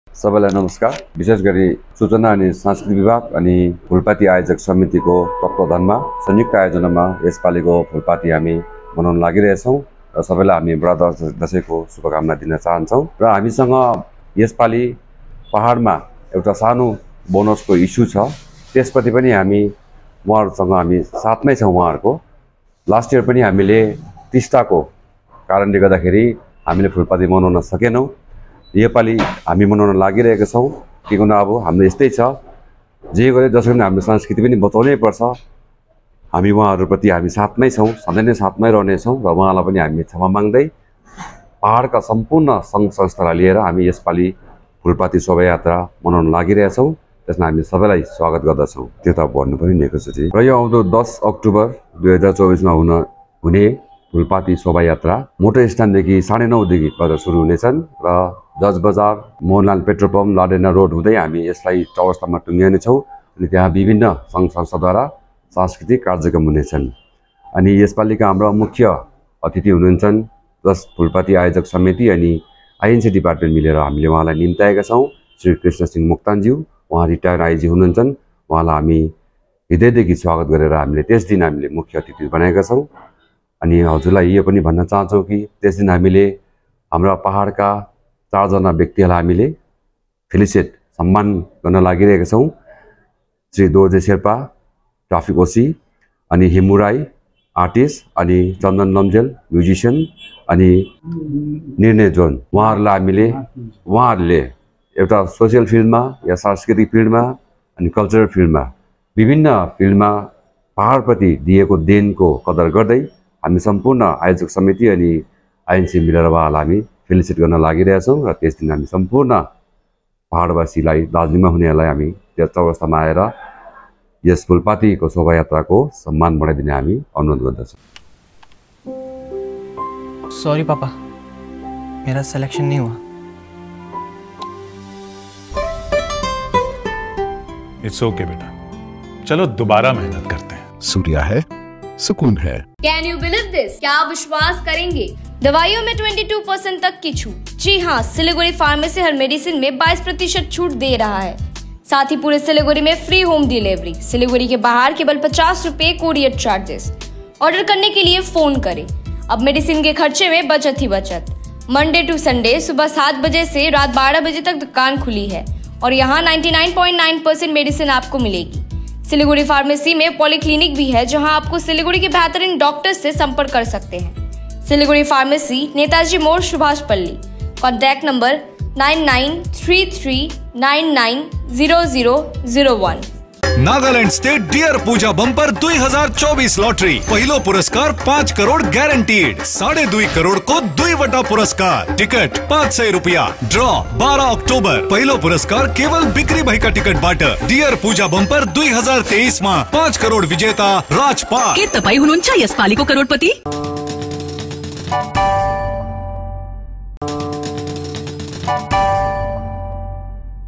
गोर्खा समुदायको पवित्र पर्व बड़ा दशैंको उपलक्ष्यमा फूलपाती शोभायात्रा लगायत विशेष कार्यक्रम
गोर्खा समुदायको पवित्र पर्व बड़ा दशैंको उपलक्ष्यमा फूलपाती शोभायात्रा लगायत विशेष कार्यक्रम यस वर्ष दिनांक १० अक्टुबर २०२४ को दिन सम्पन्न हुने